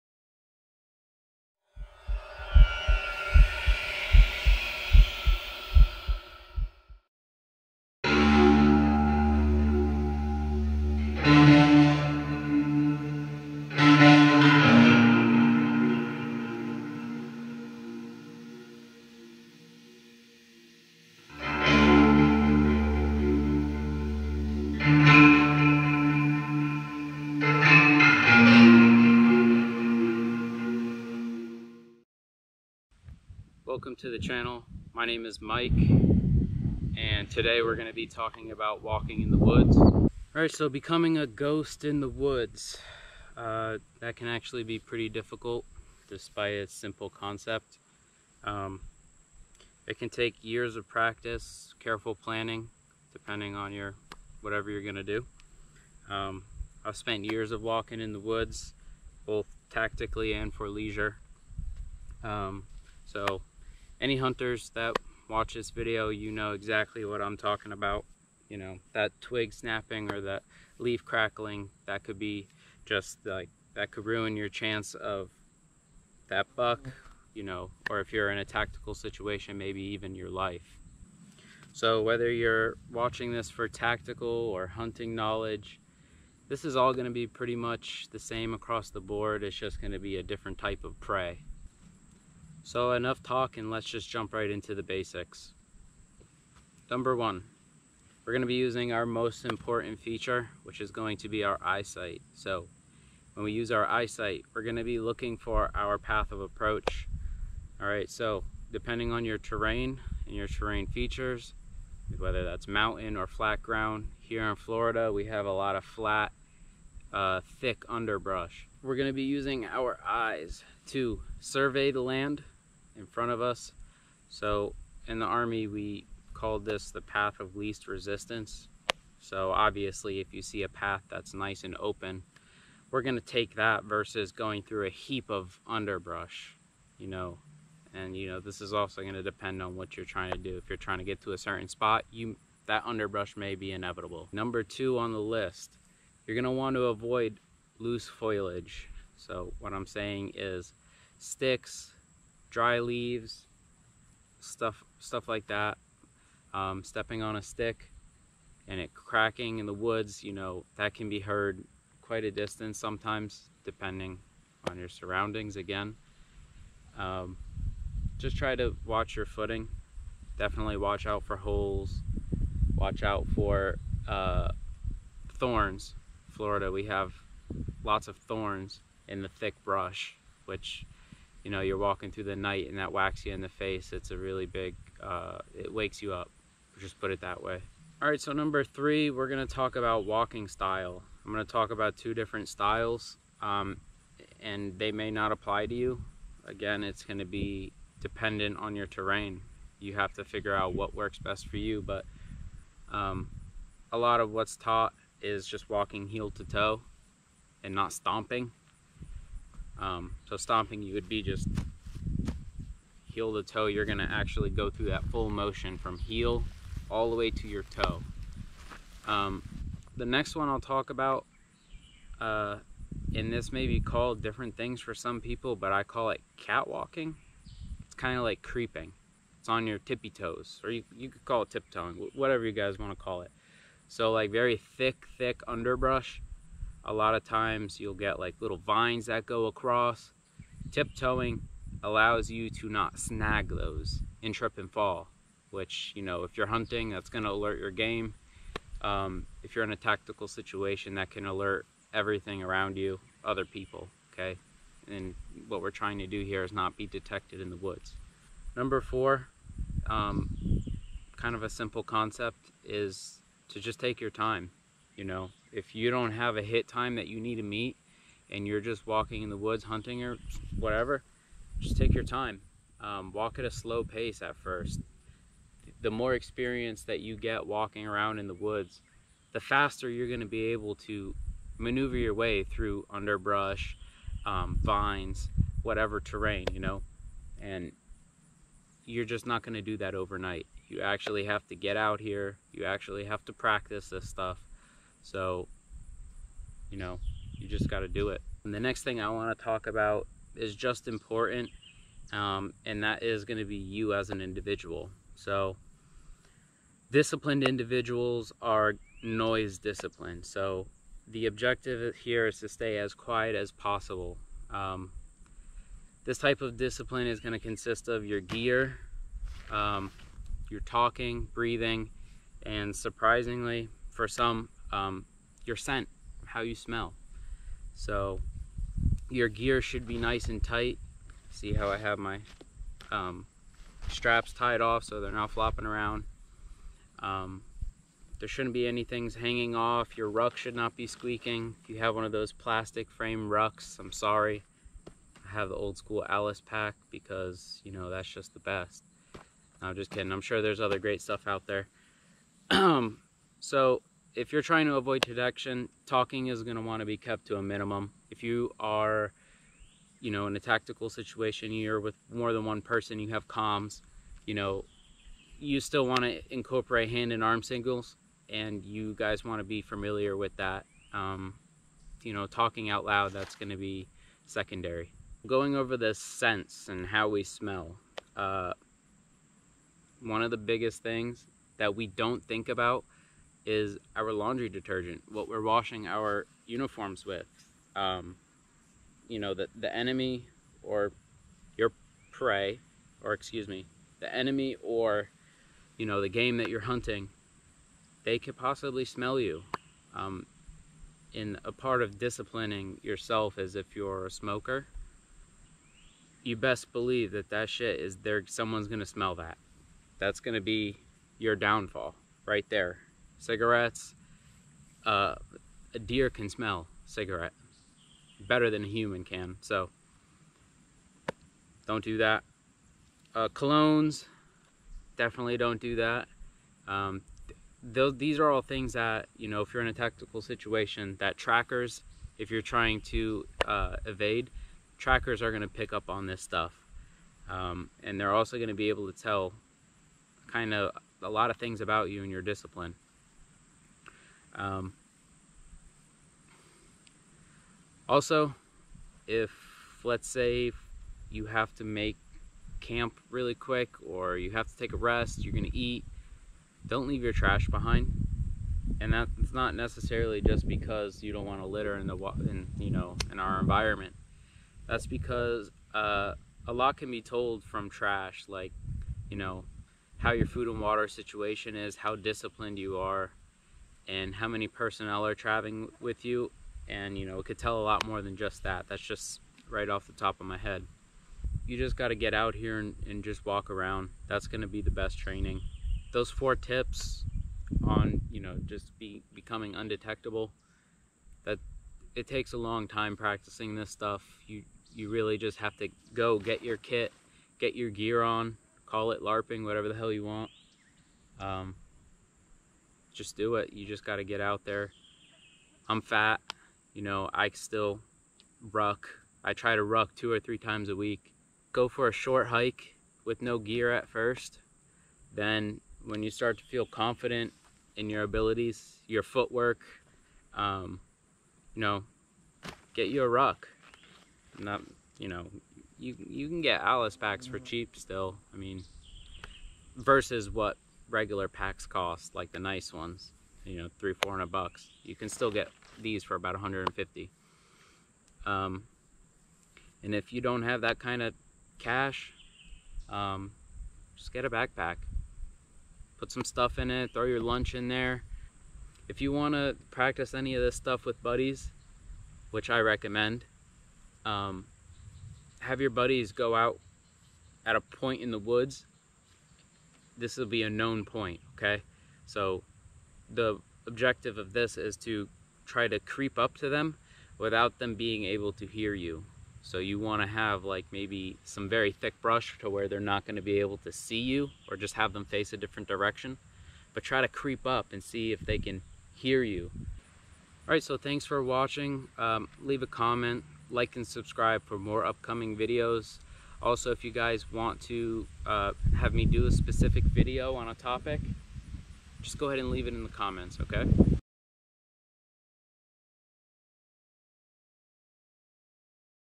Guide to Walking in the Woods Part 1
Short informational video on walking in the woods. A decent overview for entry-level hunters and tacticians. THIS IS THE BASICS and there will be more to follow.